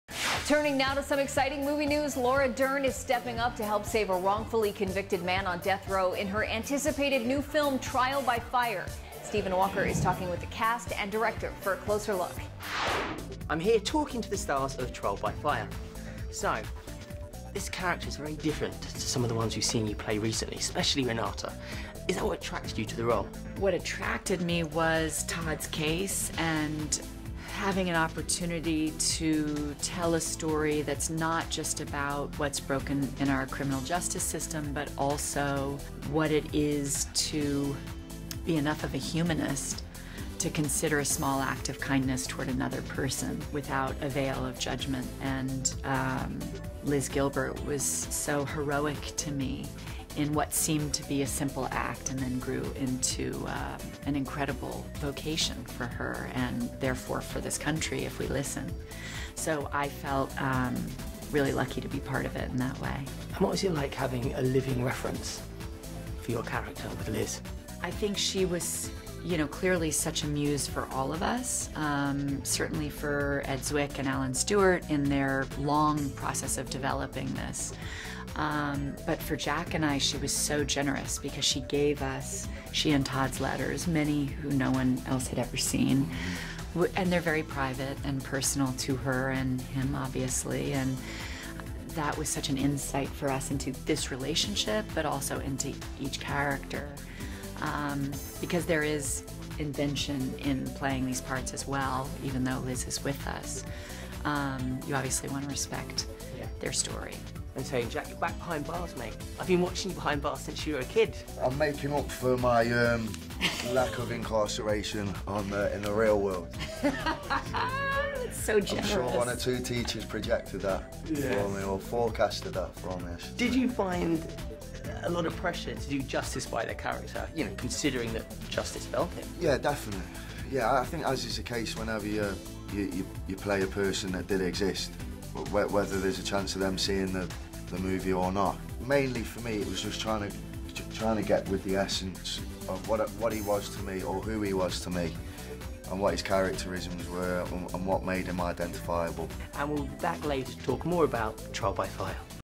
Laura Dern on Her Latest Role in 'Trial By Fire' | Celebrity Page
Our Stephen Walker is chatting with stars Laura Dern and Jack O'Connell to talk about their roles in their newest film TrialByFire! See it in theaters NOW!